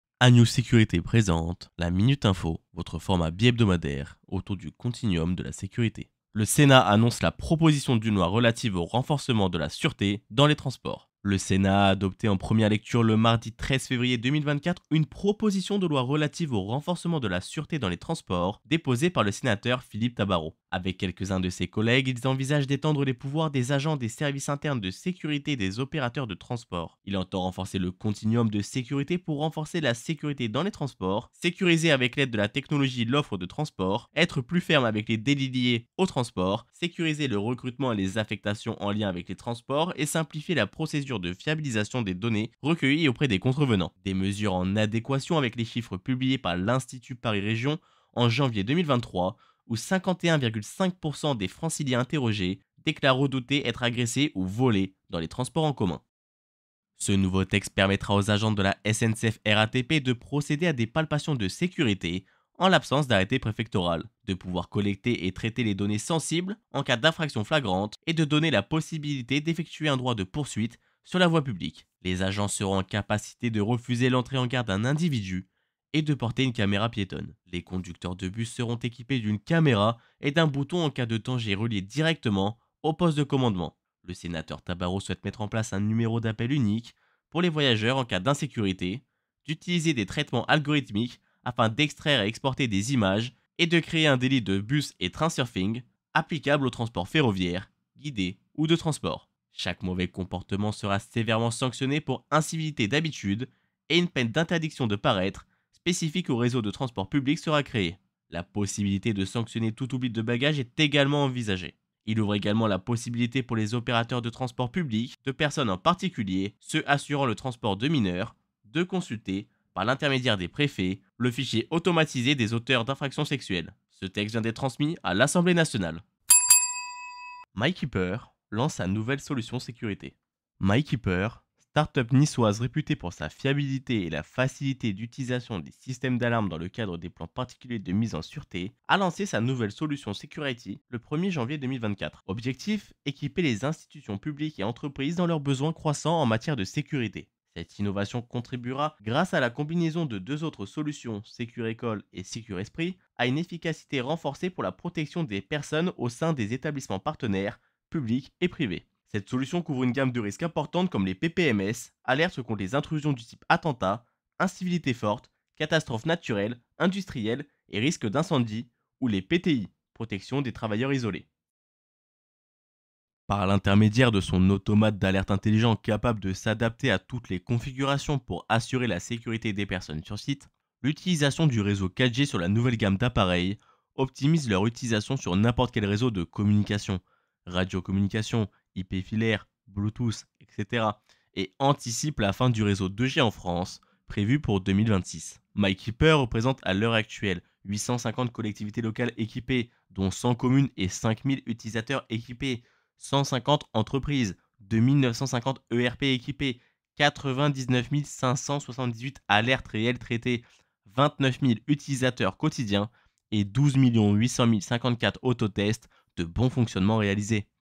La Minute Info 06
Sommaire :
- Le Sénat annonce la proposition d’une loi relative au renforcement de la sûreté dans les transports
- My Keeper lance sa nouvelle solution de sécurité

Réalisé par Baptiste Vinas

Directeur de la publication : Michaël Lejard
Chef d’édition : Eric de Riedmatten
Éditeur : Agora Médias, une activité d'Agora Managers Groupe